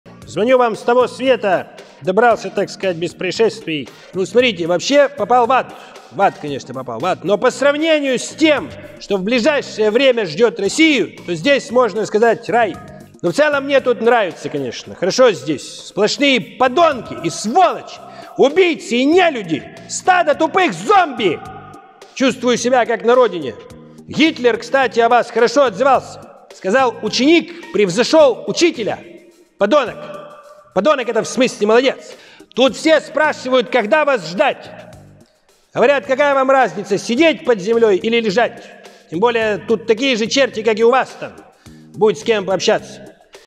Жириновский. Звонок Путину с того света 😱
Жириновский попал в ад, однако звонит Путину с рая. Как так получилось? 😁
И почему он чувствует себя там как на родине?
Такой звонок точно нельзя пропустить 😅